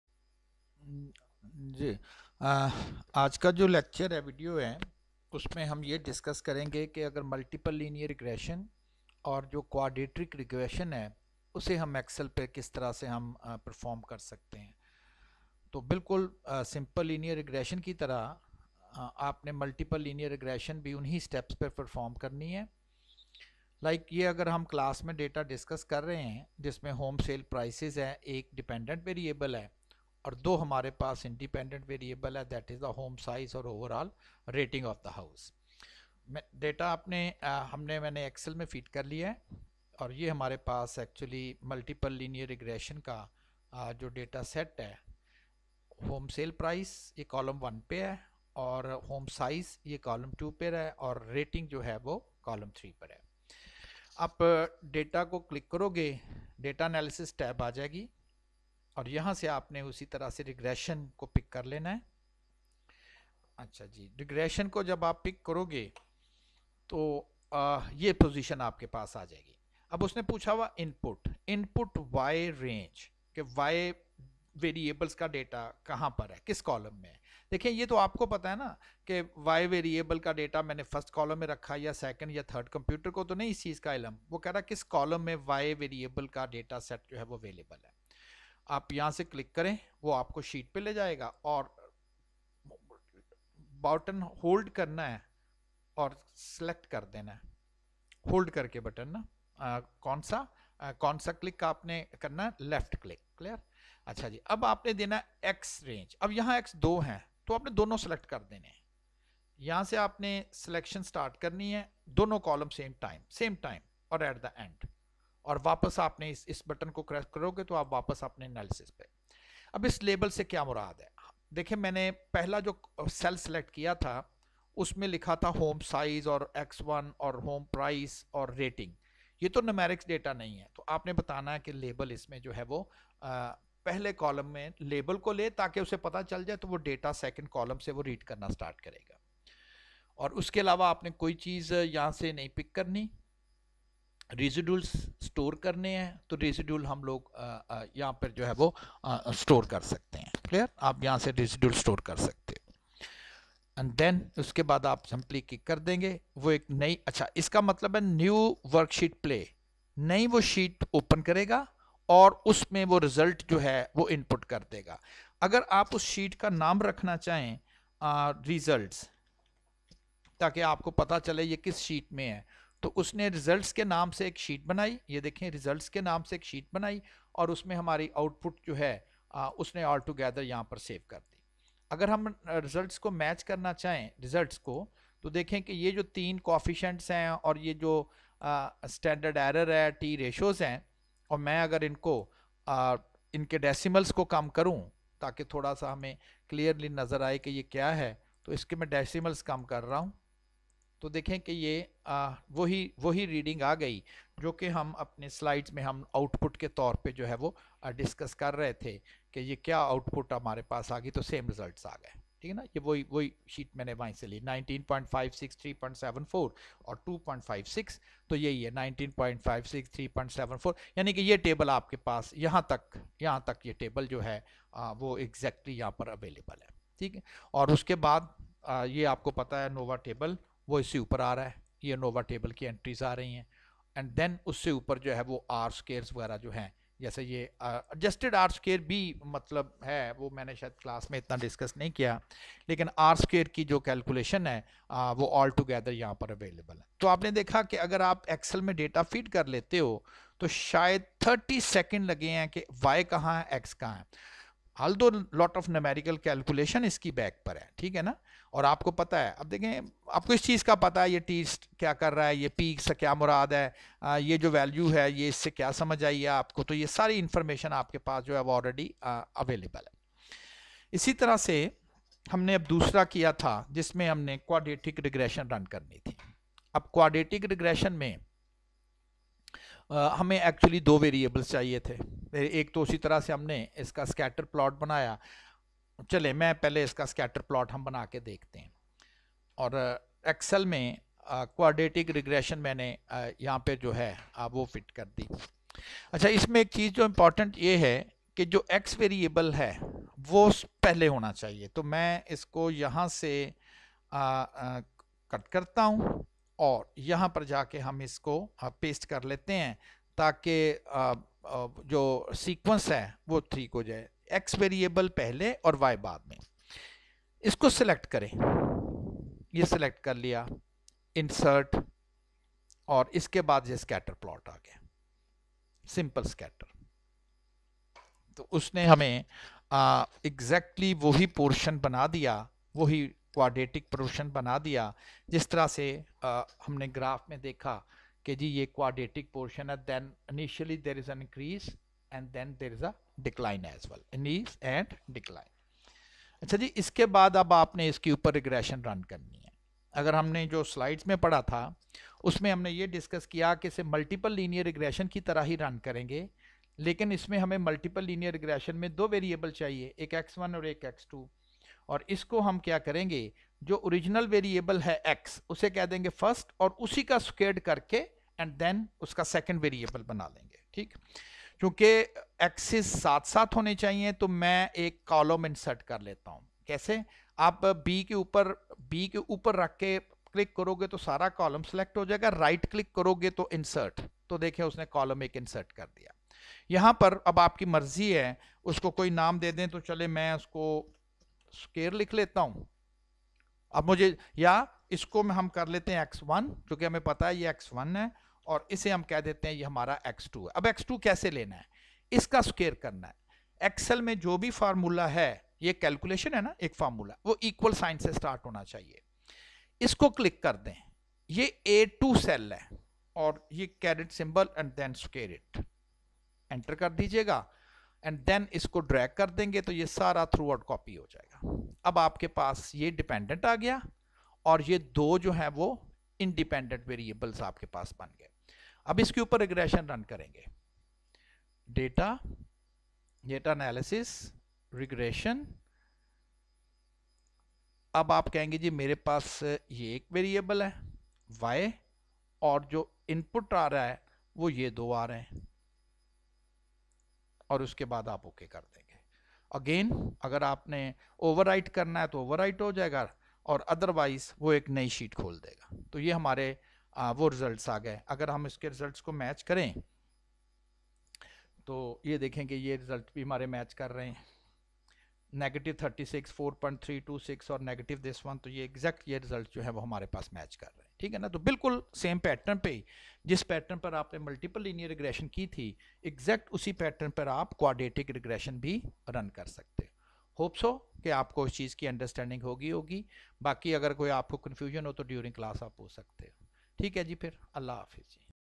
5--Multiple and Quadratic regression in EXCEL
This video explains how to perform Multiple and Quadratic regression using EXCEL